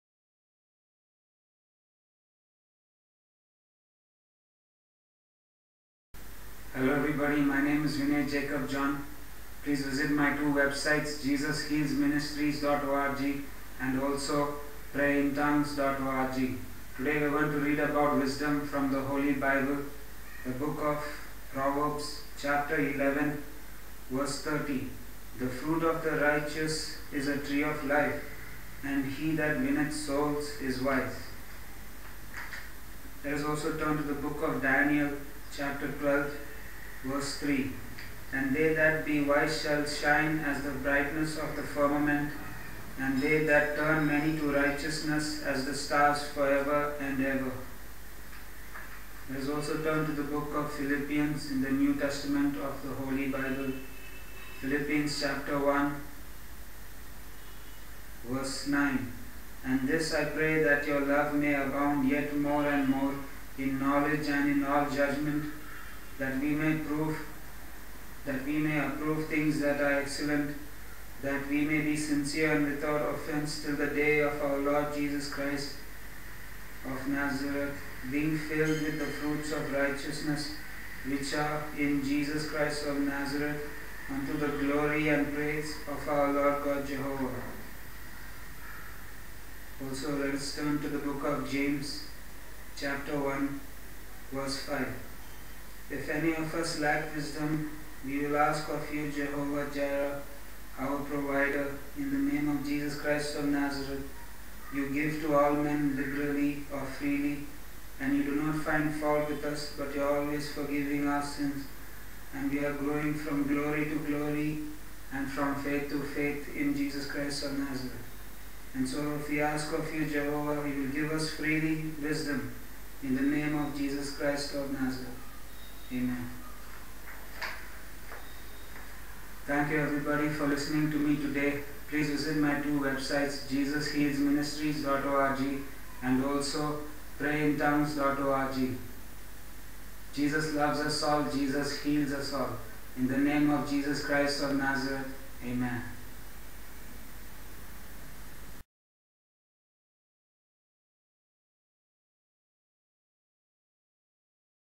Ask for Wisdom from Jesus!!!! Word for the Day!!!! - 31st Monday October 2011 !!!!
HELLO EVERYBODY HERE at Facebook and Youtube !

THIS VIDEO IS ME PROPHET   EVANGELIST   PASTOR   VINAY JACOB JOHN

READING FROM THE HOLY BIBLE FROM  -    Proverbs 11: 30

Daniel 12: 3 Philippians 1: 9 to 11 and James 1: 5, 6 !